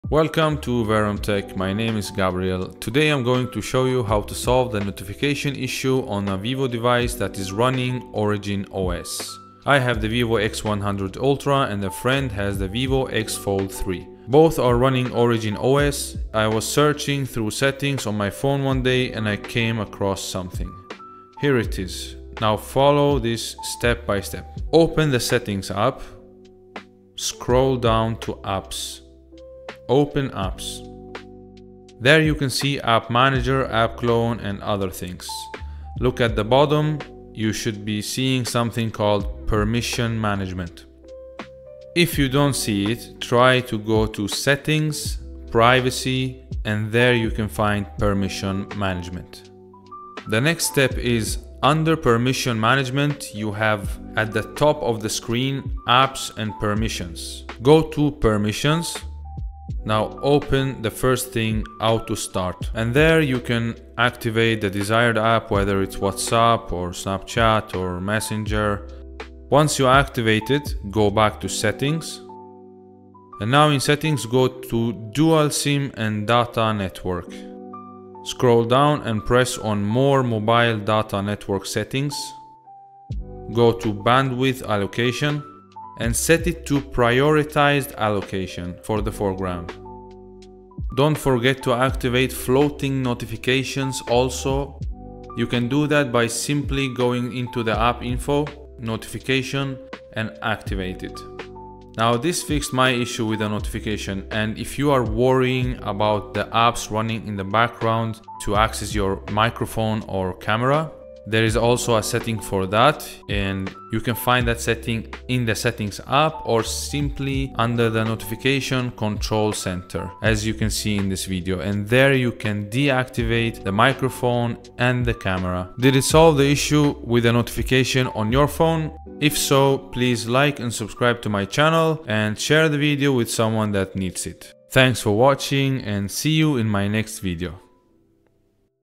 Vivo OriginOS Notification Issue [SOLVED] - WhatsApp, Telegram & All Apps.
This was bothering me for a while with Chinese phones, thankfully I found a solution and that I could share it with you all.